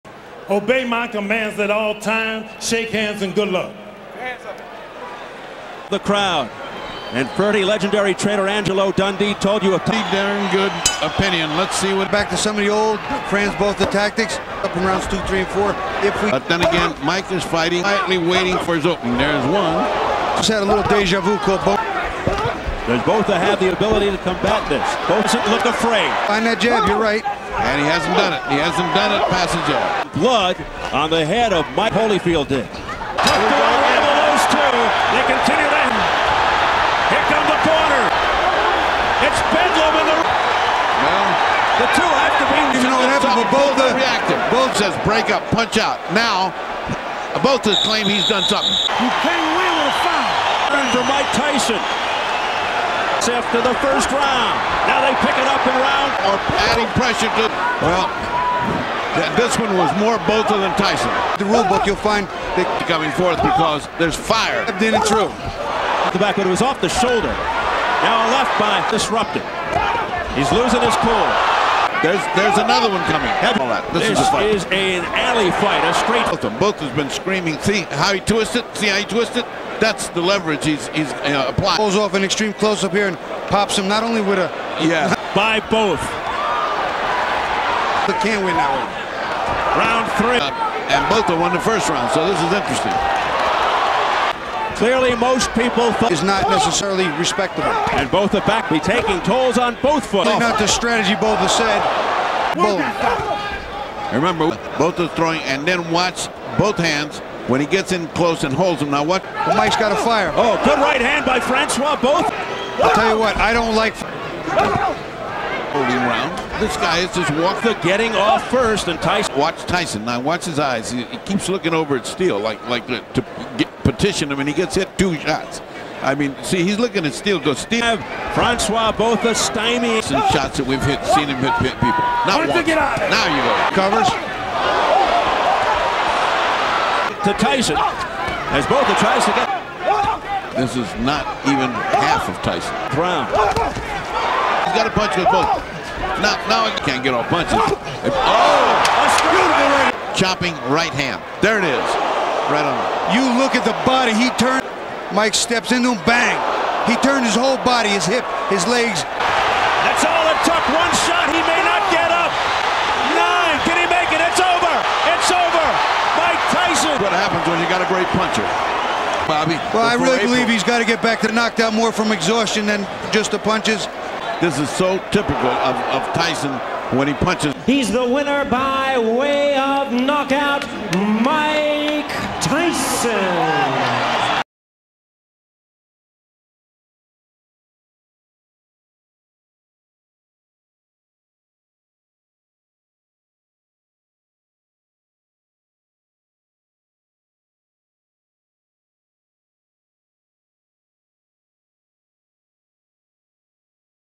Mike Tyson (USA) vs Frans Botha (South Africa) | Boxing Highlights | HD
Watch the thrilling highlights of the bout between Mike Tyson and Frans Botha in stunning HD! Witness Tyson's ferocious power and relentless aggression as he takes on the tough South African fighter, Botha, in a match filled with intense action and dramatic moments. Relive the key exchanges and unforgettable highlights that showcase Tyson's legendary prowess in the ring.